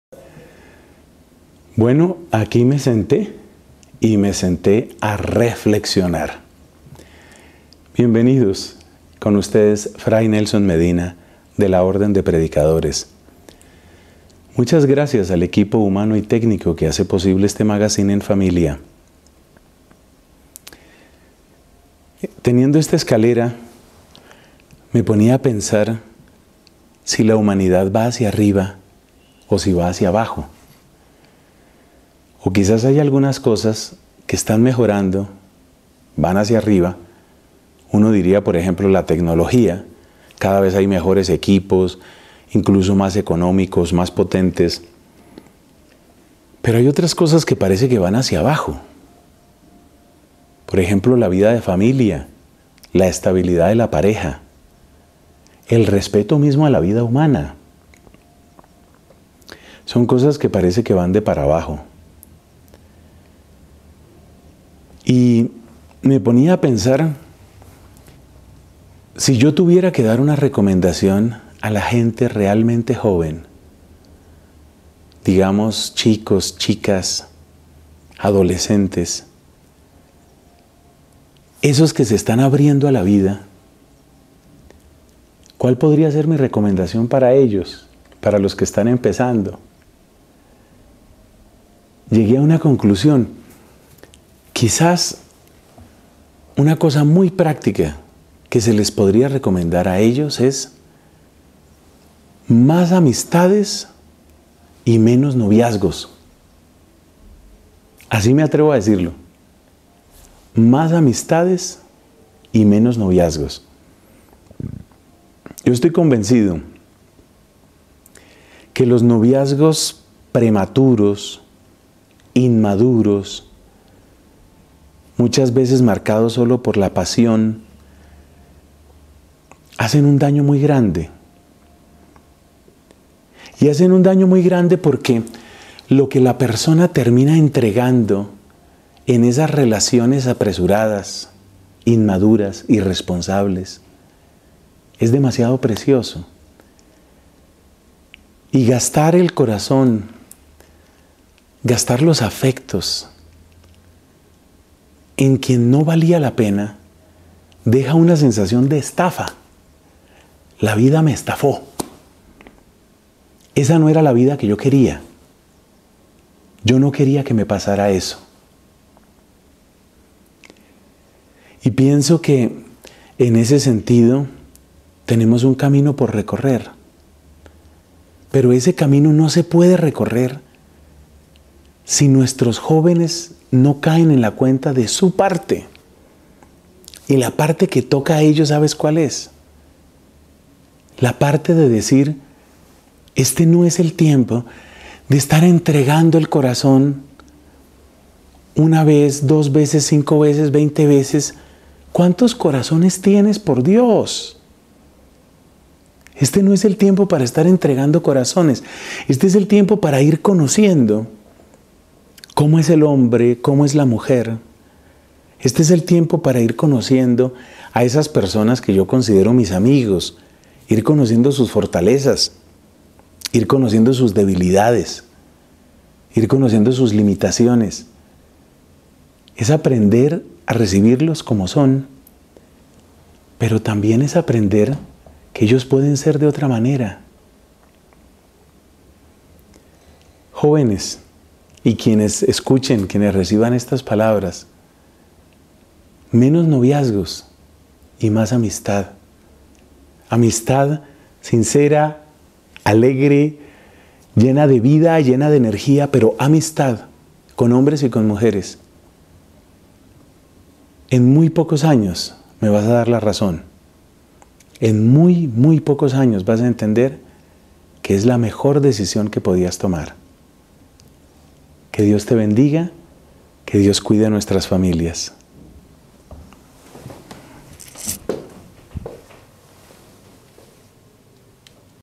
ENFAMILIA 11-11, Mas amistades y menos noviazgos!
Los noviazgos prematuros, inmaduros y llenos de pasión son una receta perfecta para destruir la dignidad y a autoestima de todos, sobre todo de las mujeres. mucho mejor opción es cultivar el don de la amistad con hombres y con mujeres, y dar tiempo al tiempo.